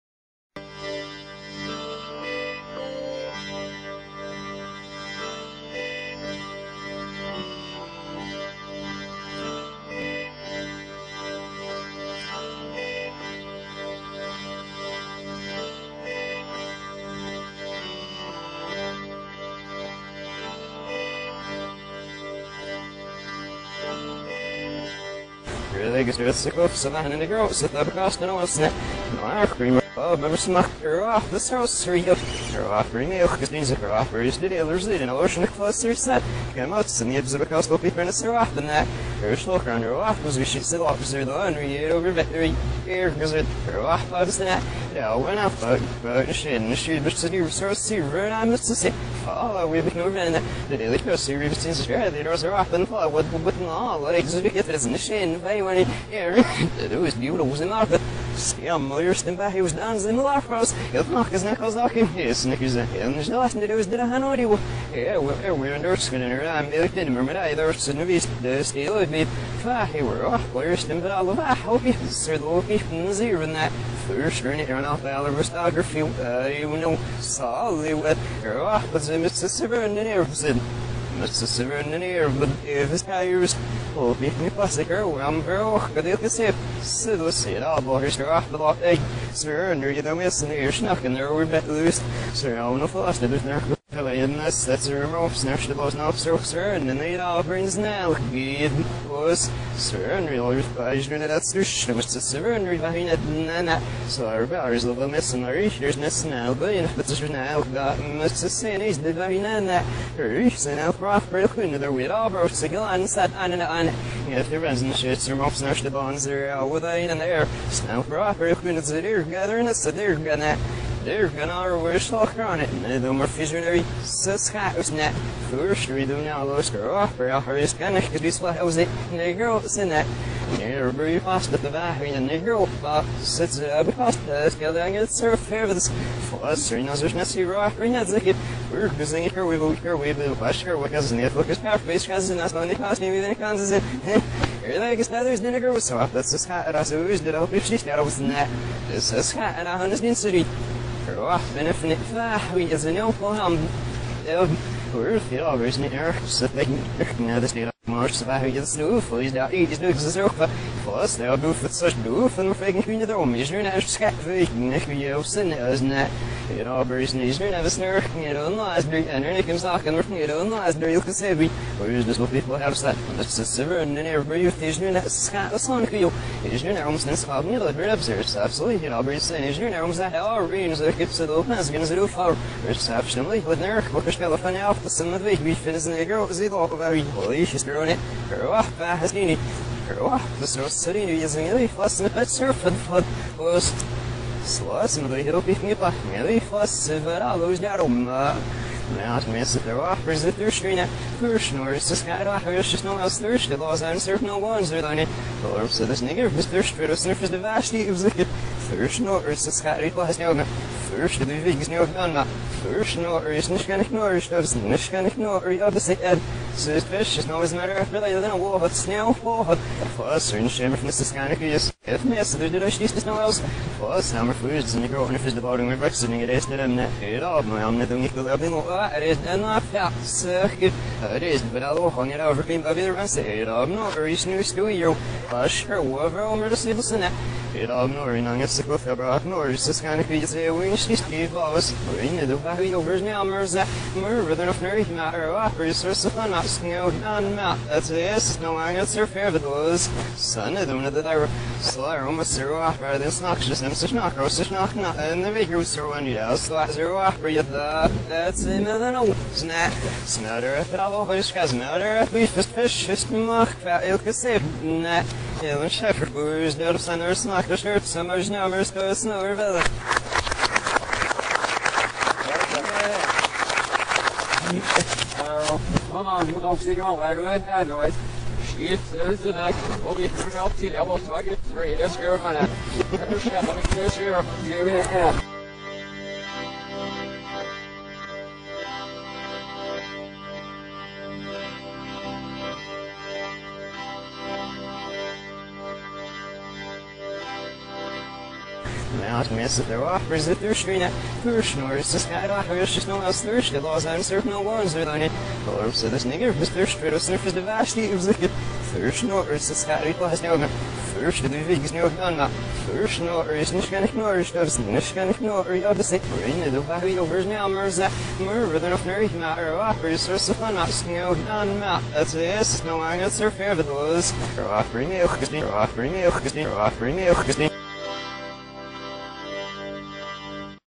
REVERSED - Human Light Technology Part 1
*many people believe that reversing forward speech can reveal messages otherwise hidden from the conscious mind

video response to:
Human Light Technology Part 1

Metaphors commonly found in reverse speech can be found in
The RS Dictionary thread here:

under reversing tools and methods

evp reversed speaking
peggy
kane
nindica
pinion
nindicapinion
light
vibration
sound